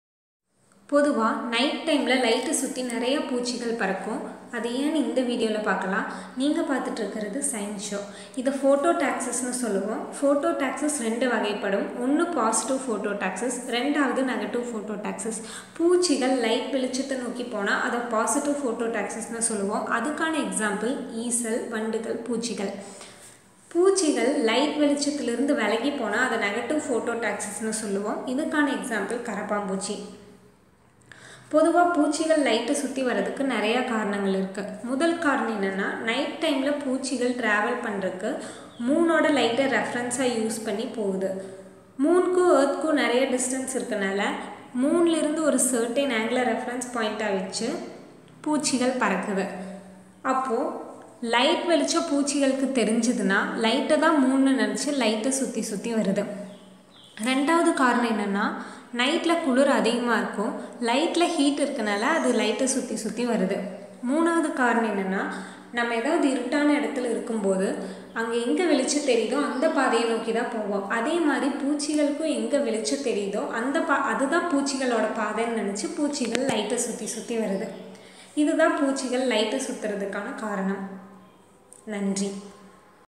Why insects attract towards the light?
Why insects attract towards the light? explained in tamil.